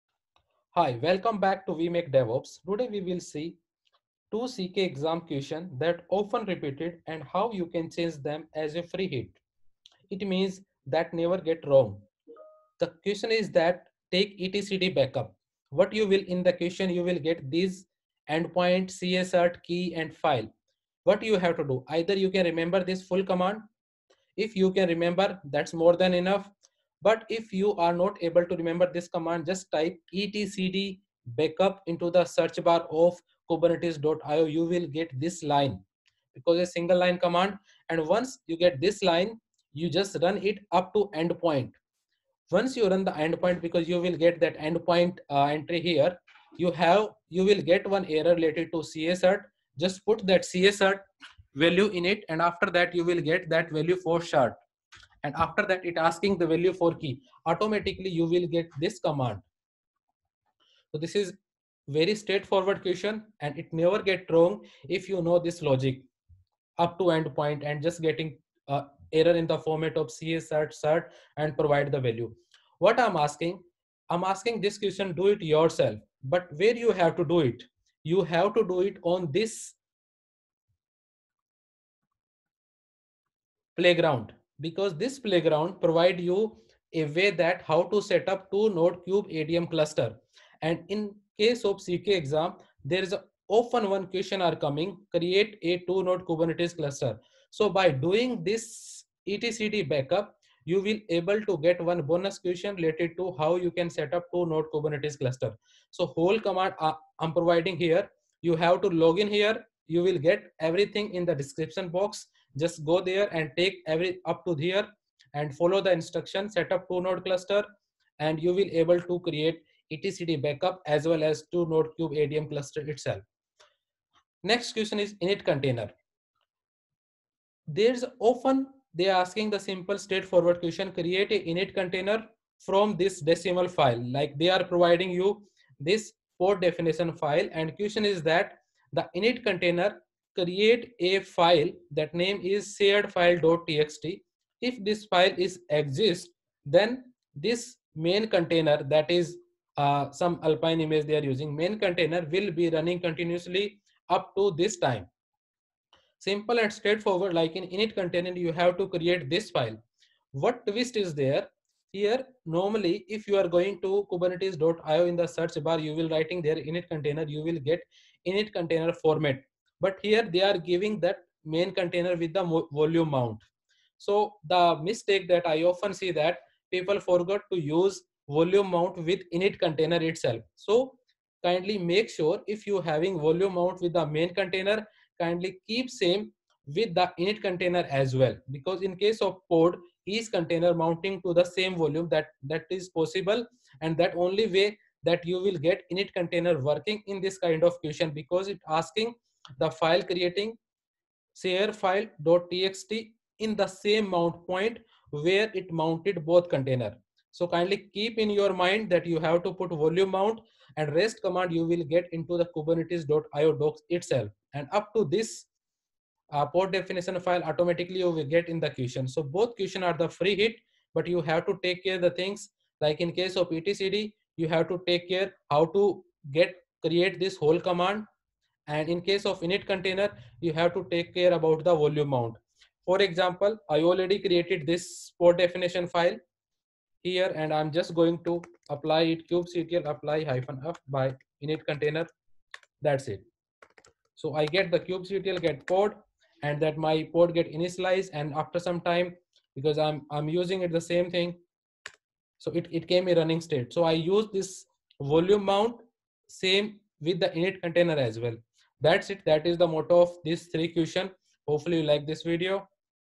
Kubernetes Certification- Crack the CKA with WeMakeDevOps
1)

cacert=/etc/kubernetes/pki/etcd/ca.crt
key=/etc/kubernetes/pki/etcd/server.key
File Path=/tmp/test.rb

ETCDCTL_API=3 etcdctl --endpoints=[ENDPOINT] --cacert=[CA CERT] --cert=[ETCD SERVER CERT] --key=[ETCD SERVER KEY] snapshot save [BACKUP FILE NAME]

ETCDCTL_API=3 etcdctl --endpoints $ENDPOINT snapshot save snapshotdb

ETCDCTL_API=3 etcdctl --endpoints= --cacert=/etc/kubernetes/pki/etcd/ca.crt --cert=/etc/kubernetes/pki/etcd/server.crt --key=/etc/kubernetes/pki/etcd/server.key snapshot status /tmp/test.rb

DIY:

( Register it required )
2) Select and setup 2 node k8s Kubeadm cluster ( you will get instruction on the first node itself )
3) Run some test deployment
4) Setup ETCD client as per below guid ( might be it required to install wget via yum)

yum install wget -y

tar xvf etcd-v${RELEASE}-linux-amd64.tar.gz
cd etcd-v${RELEASE}-linux-amd64
sudo mv etcd etcdctl /usr/local/bin

2)

Bonus questions: By doing above exercise you will get how to setup 2 node kubernetes cluster

3)
We need to use Init Container to create a file named “sharedfile.txt”
under the “work” directory. Here application container should check if the file exists and sleep for a while.
If the file does not exist the application container should exit.

Questions: ( Answer i will put in first comment)

apiVersion: v1
kind: Pod
metadata:
  name: init-container-test
spec:
  containers:
  - name: application-container
    image: alpine
    command: ['sh', '-c', 'if [ -f /work/sharedfile.txt ]; then sleep 99999; else exit; fi']
    volumeMounts:
    - name: workdir-volume
      mountPath: /work
  volumes:
  - name: workdir-volume
    emptyDir: {}